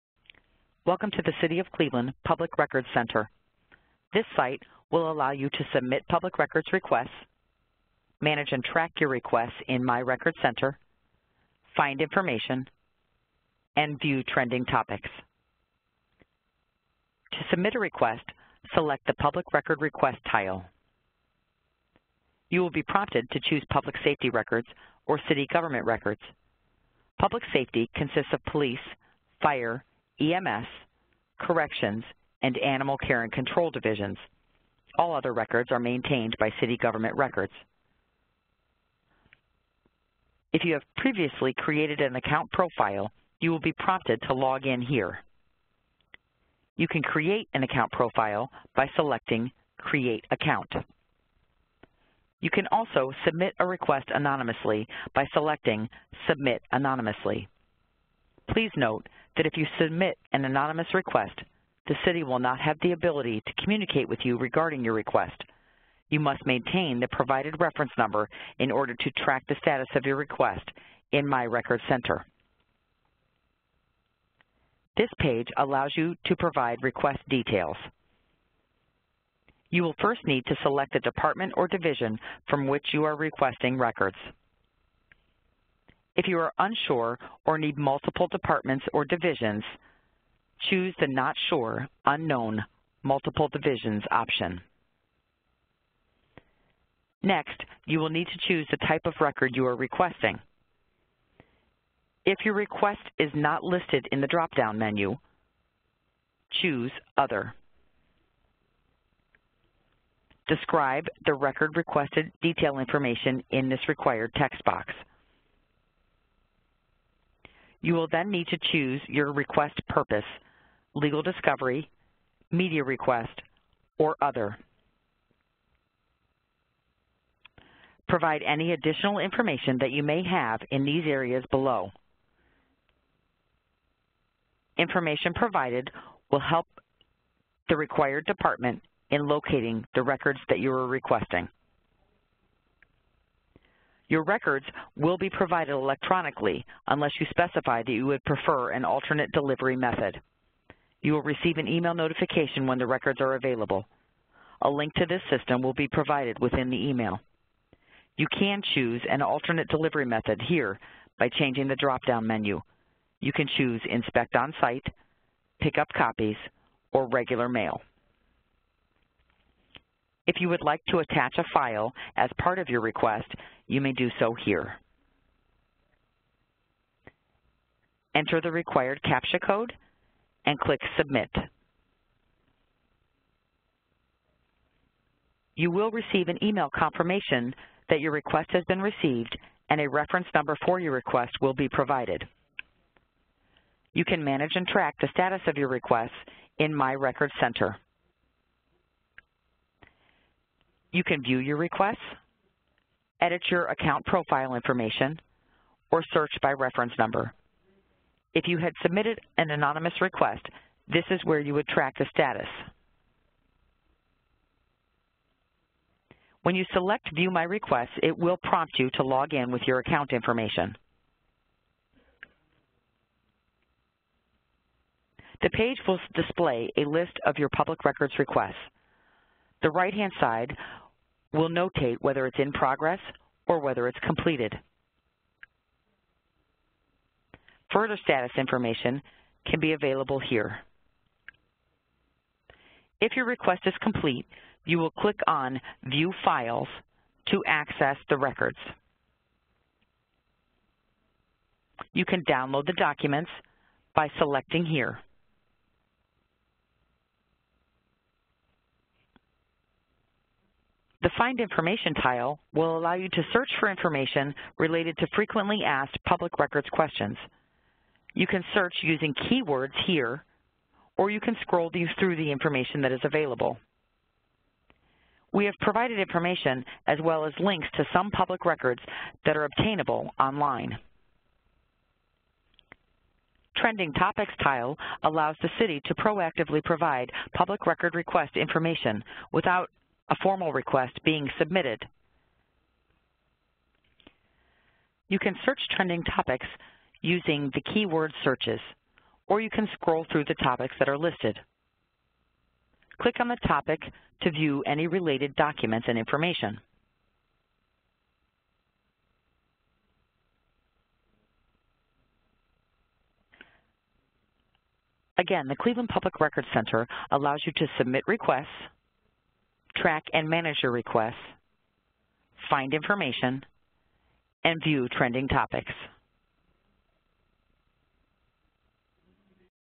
City Of Cleveland Public Records Center Tutorial
A step by step guide to the new City Of Cleveland Public Records Center.

TV20, We Are Cleveland!